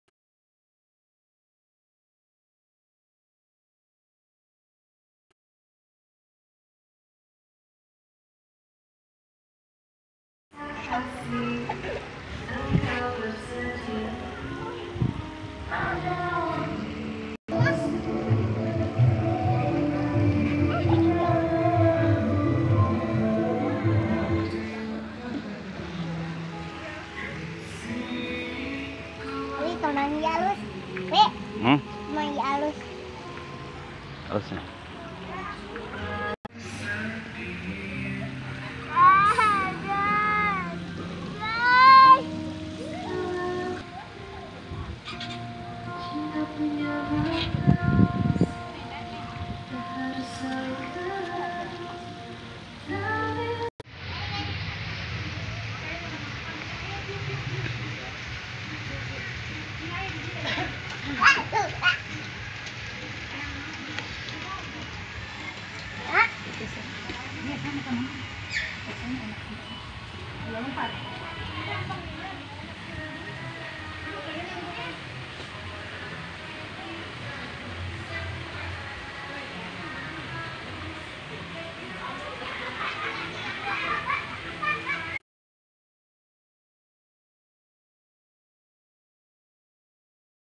KULINER INDONESIA‼️BAKSO RAKSASA‼️ bakso viral‼️
Kuliner‼️kuliner Karawang di antarnya yaitu Bakso Raksasa Merupakan Salasatu Bakso yang enak lezat halal yang ada di karawang,lokasi bakso raksasa berada di jalan syekh quro telagasari karawang,harga bakso raksasa telagasari ini cukup terjangkau,menu bakso yang tersedia ada macam macam jenis bakso yang di tawarkan di sini
kulinerkarawang
kulinerkarawangterkenal
baksoraksasa
baksoraksasapedas
baksokarawang
kulinerkarawang
kulinerkarawangviral
baksoraksasakulinerkarawangpalingviral
Di dokumentasi kan oleh channel YouTube channnel yabunayya miftahul amal kiara